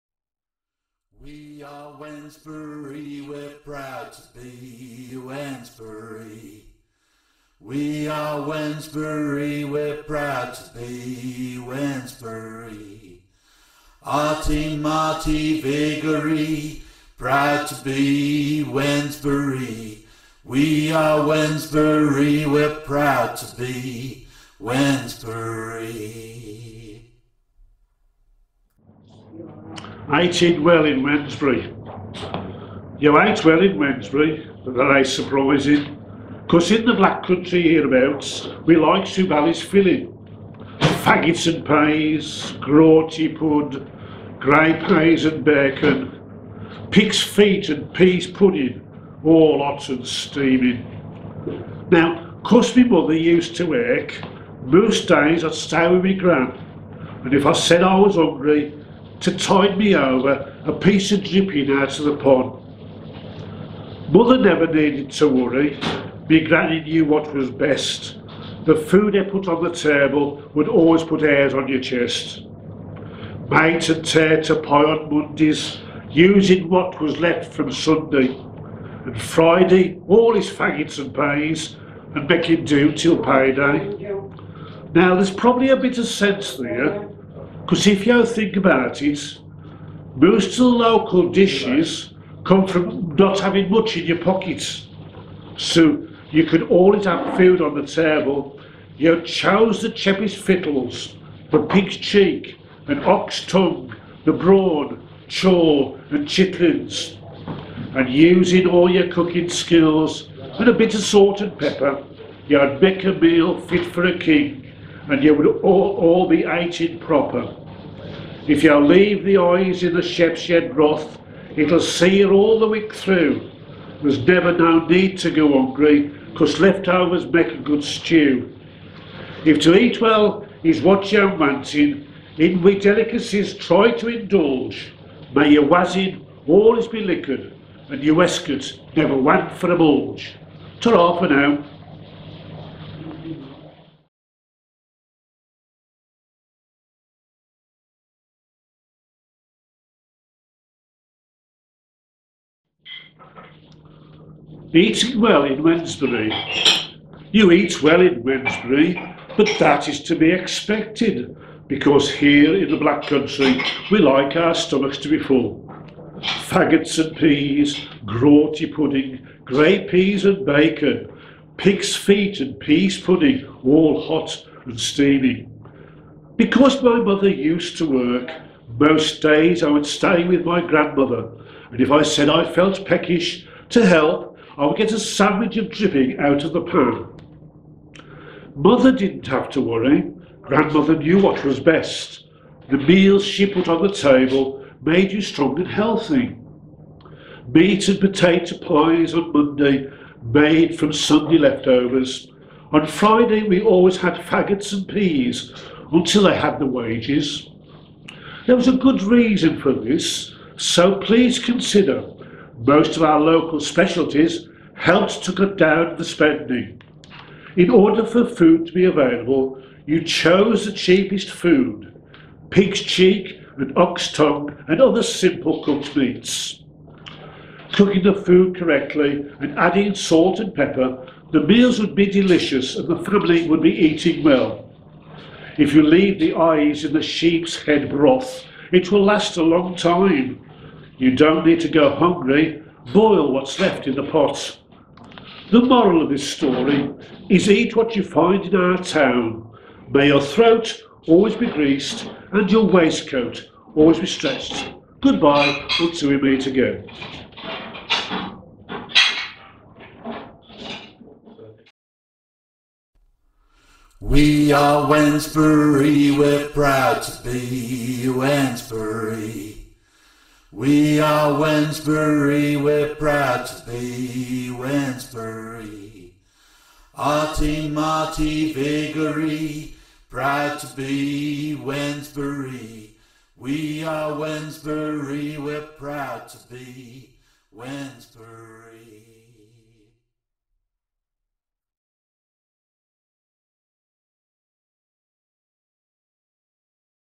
Poetry / Aytin' Well in Wednsbry by Keith Gwilliams / Street Full of Dialect / We Are Wednesbury
Aytin' Well in Wednsbry is a newly commissioned poem written and performed by Keith Gwilliams.

The poem was commissioned in 2021 as part of A Street Full of Dialect, a project produced by Brendan Hawthorne as part of the wider We Are Wednesbury cultural programme funded by Historic England and Sandwell Council, and produced by Multistory.